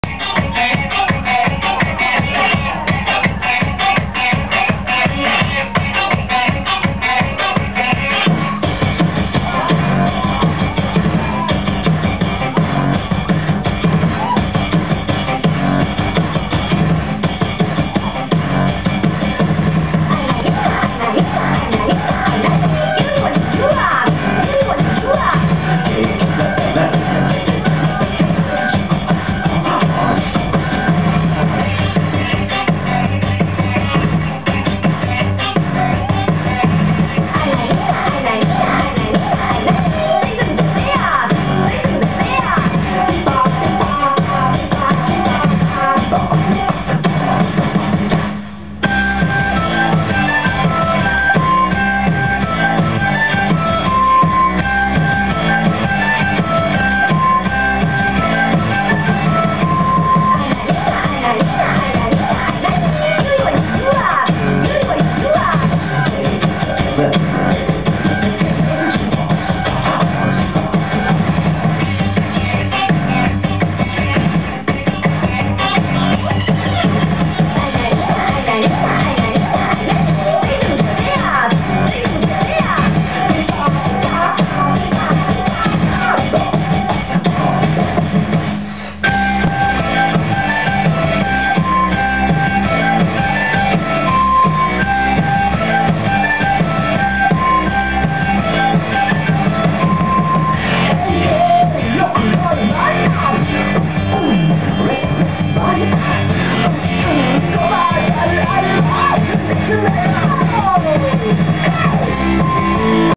Japanese Robot dance at Union
Japanese kids dancing the robot awesome!